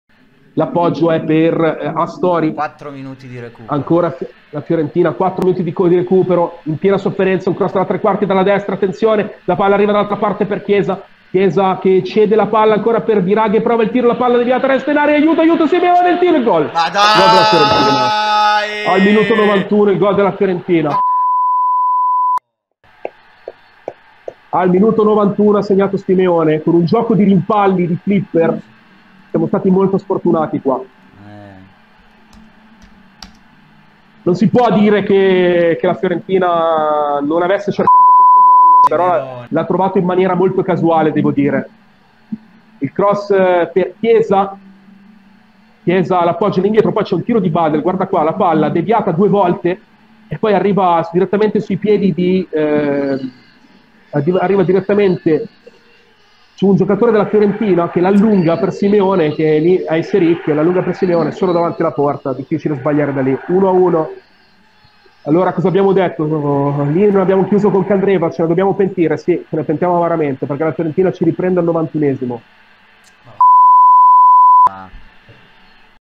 Gol Simeone in Fiorentina-Inter | LIVE REACTION
Ecco la live reaction al gol di Simeone! Cronaca di Fulvio Santucci e Massimo Suppo

In collaborazione con le pagine Facebook:

Il Malpensante - L'Inter come non te la racconterà mai nessuno.